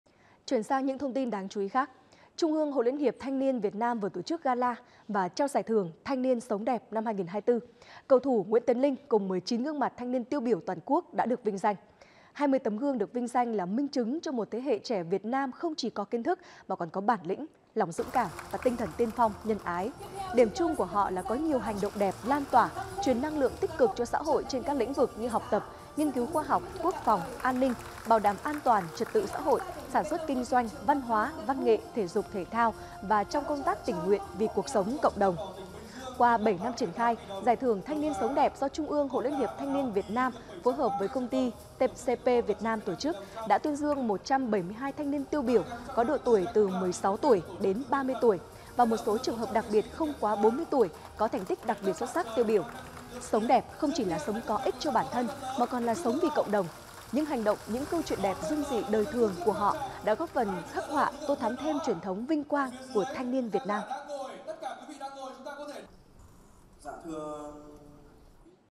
Tôn vinh 20 tấm gương “Thanh niên sống đẹp” năm 2024 | Tin tức
Chiều 16/10, tại Hà Nội, Trung ương Hội Liên hiệp Thanh niên Việt Nam đã tổ chức Gala và trao Giải thưởng “Thanh niên sống đẹp” năm 2024. Cầu thủ Nguyễn Tiến Linh cùng 19 gương mặt thanh niên tiêu biểu toàn quốc đã được vinh danh.